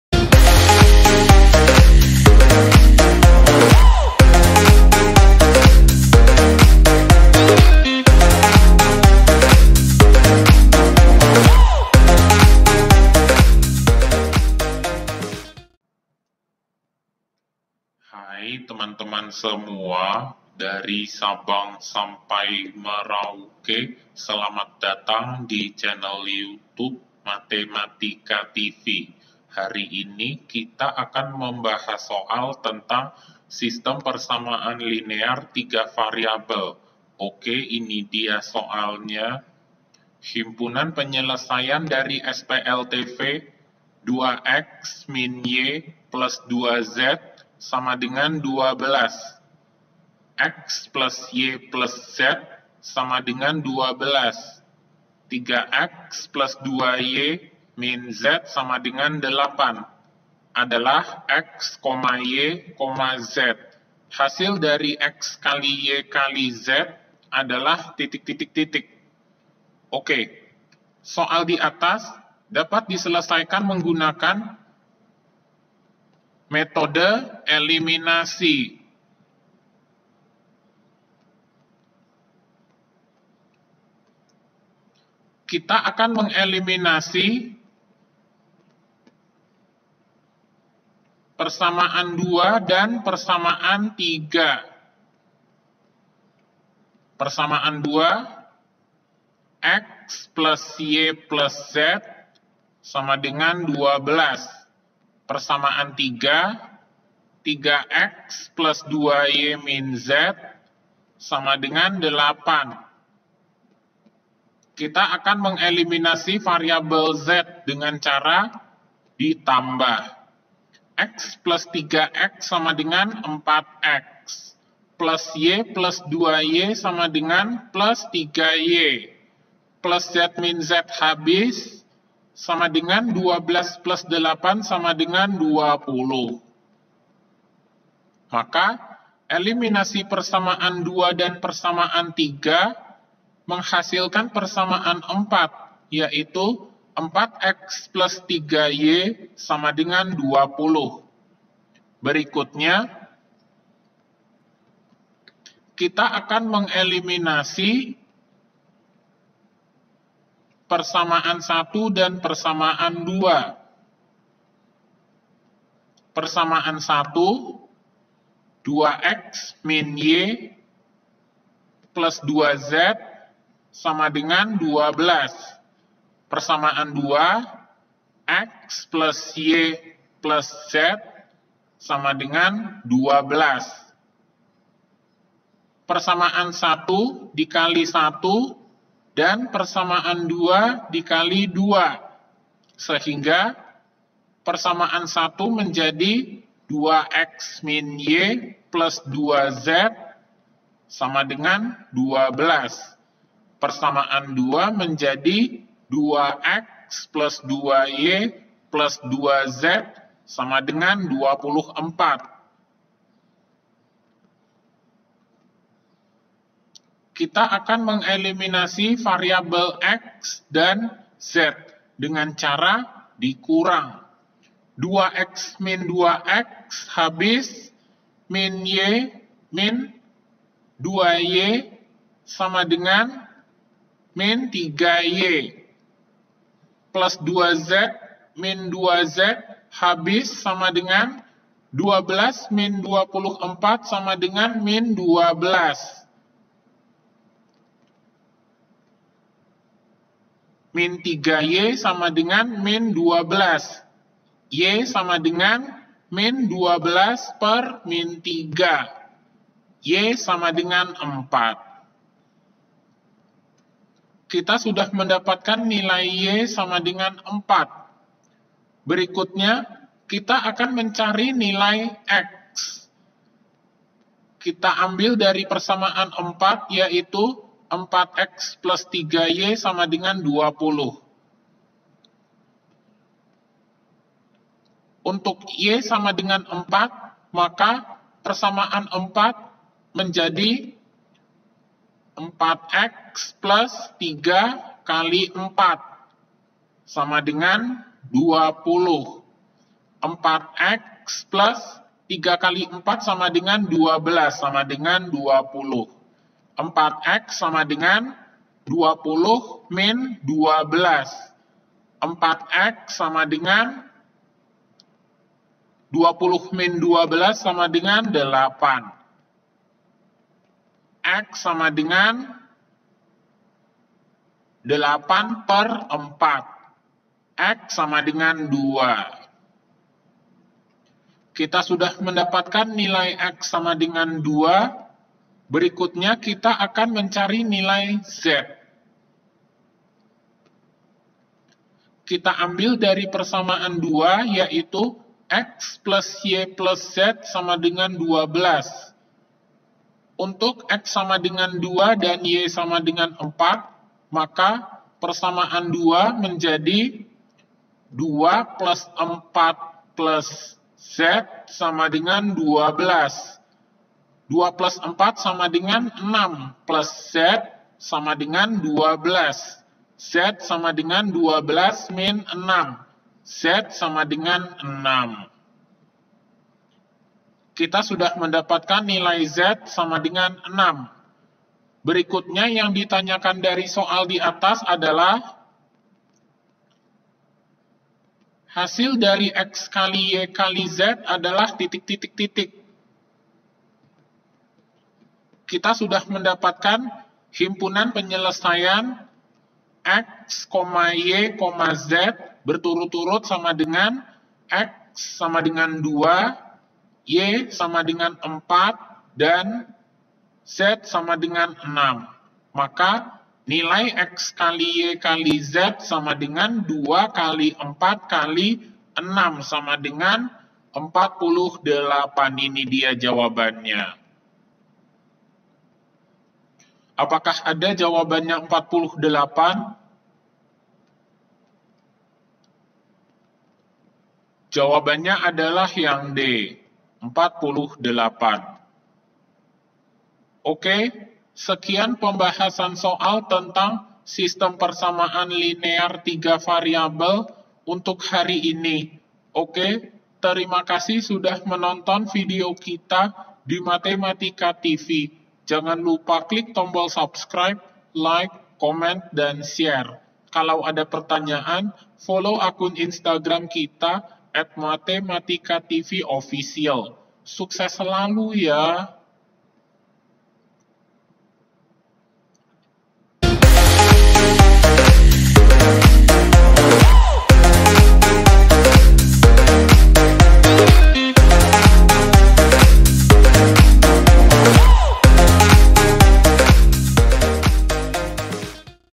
[KELAS X WAJIB - KURIKULUM 2013] PEMBAHASAN SOAL PG SISTEM PERSAMAAN LINEAR 3 VARIABEL (011)
Pada kesempatan kali ini kita akan membahas soal cerita yang berkaitan dengan sistem persamaan linear 3 variabel.